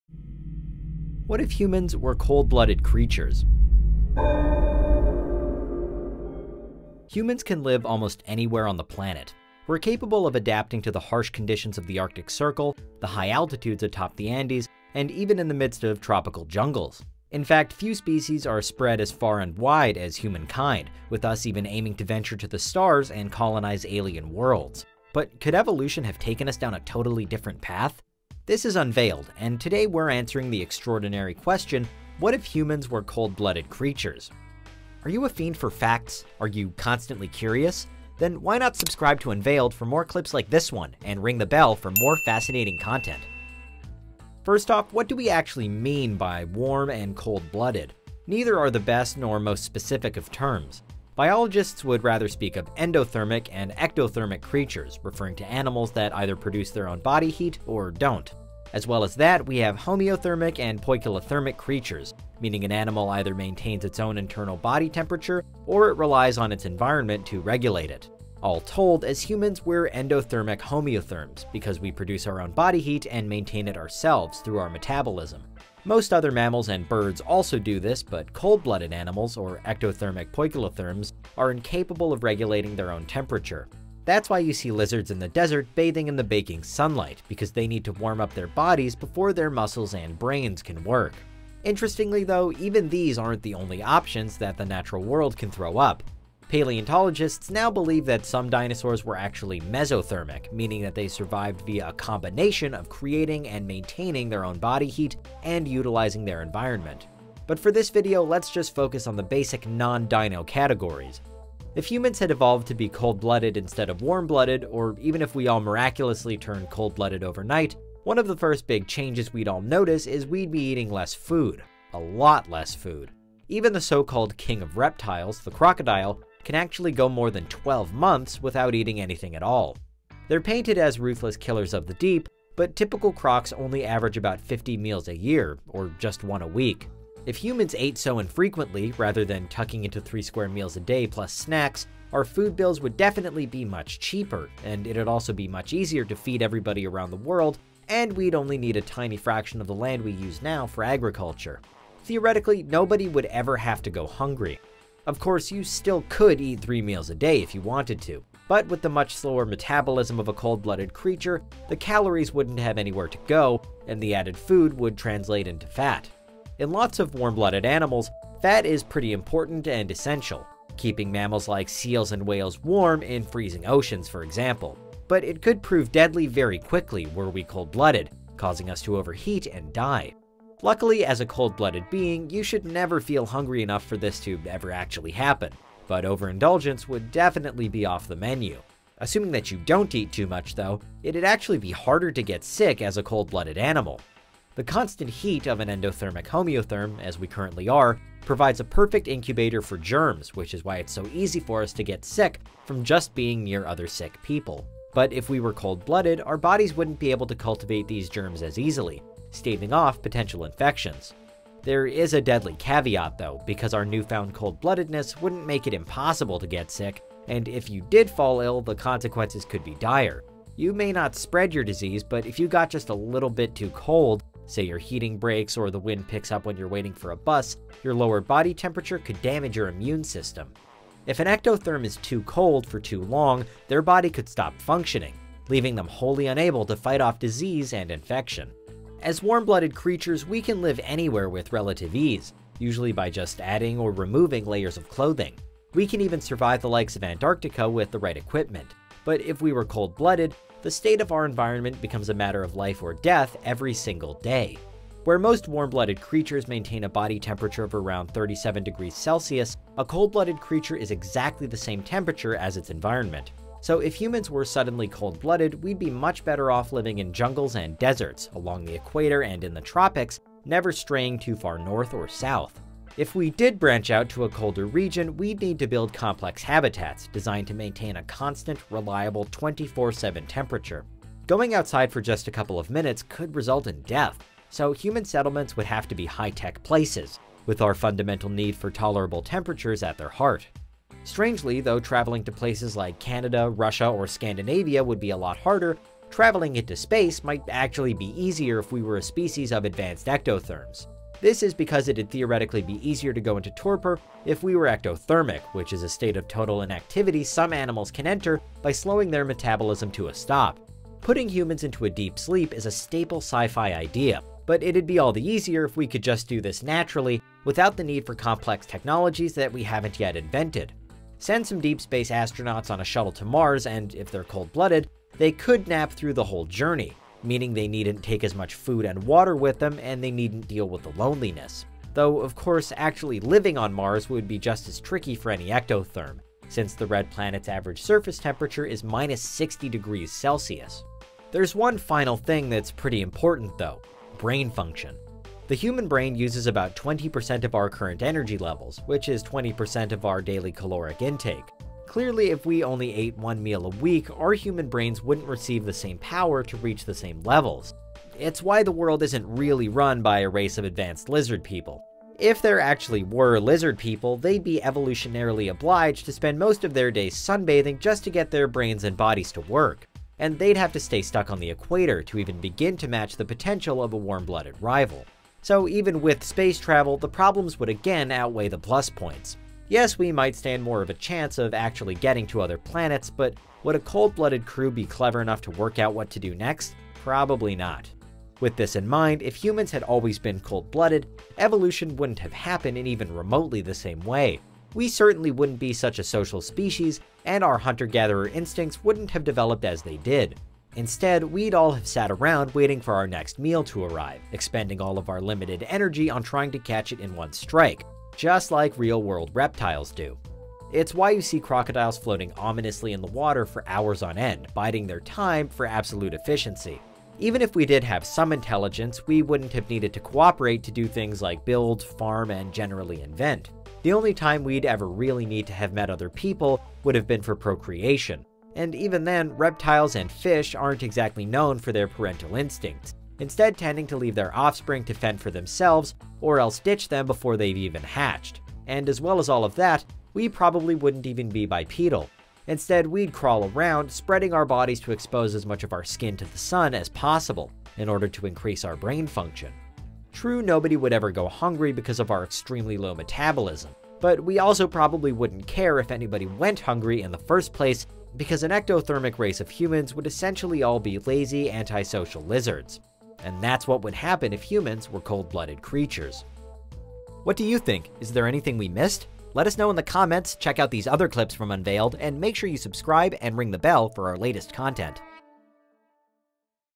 What If Humans Were Cold-blooded Creatures? | Unveiled
Human beings are warm-blooded mammals. It's a fact of basic science. But, what if they weren't? How would our lives be different if we were actually cold-blooded? In this video, Unveiled discovers how our bodies would change for a cold-blooded lifestyle, the dangers we'd face but also the surprising benefits we'd suddenly enjoy. Scientifically speaking, it's about more than just an advanced race of hyper-intelligent lizard people... What do you think would happen if humans were cold-blooded?

Are you constantly curious? Are you a fiend for facts?